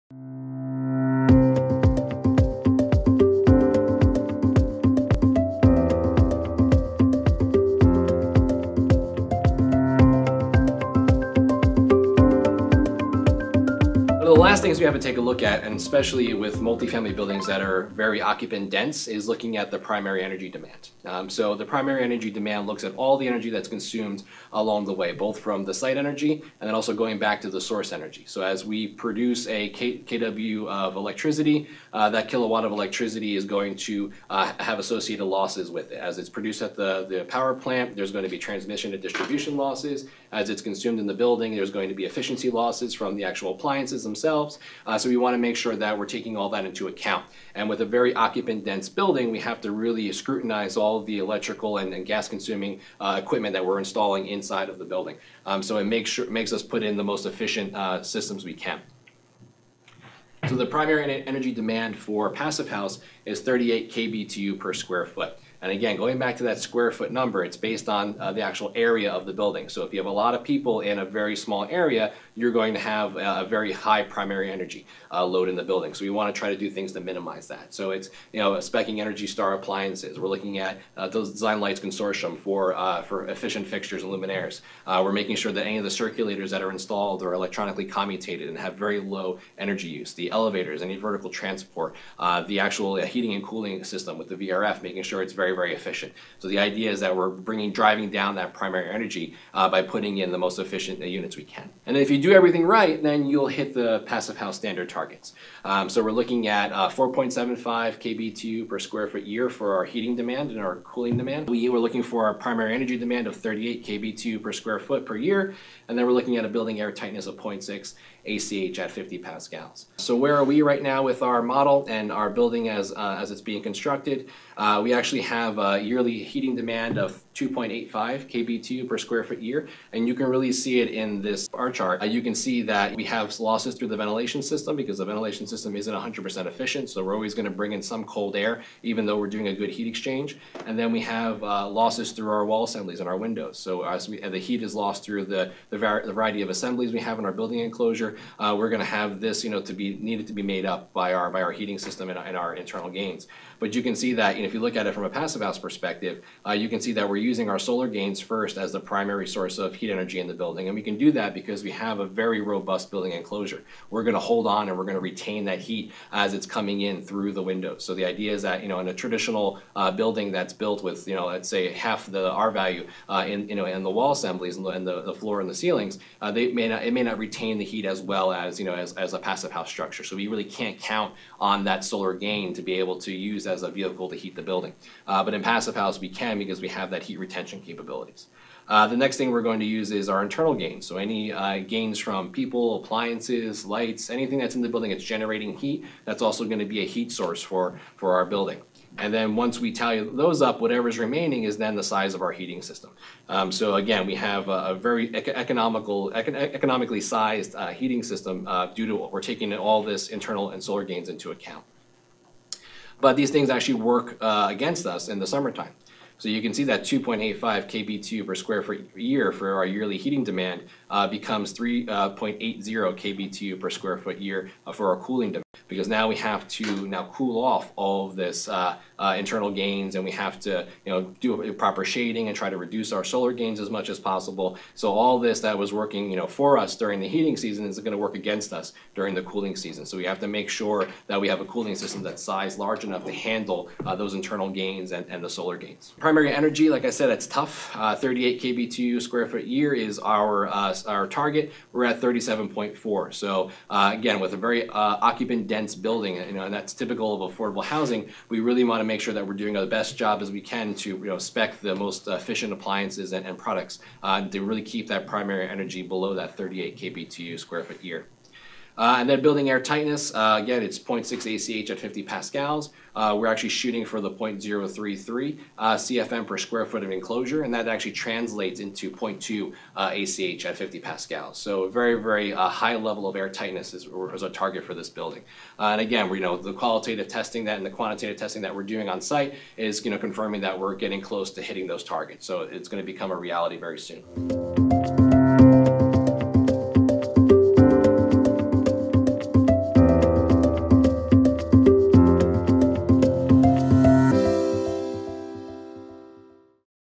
6 PH Principles: Other Key Design Elements and Considerations
A look at all the factors contributing to primary energy demand at Corona Senior Residence. and how the project is balancing internal and solar gains against transmission and distribution losses, ventilation losses and more throughout the heating and cooling seasons, and the role of equipment selection in achieving this balance.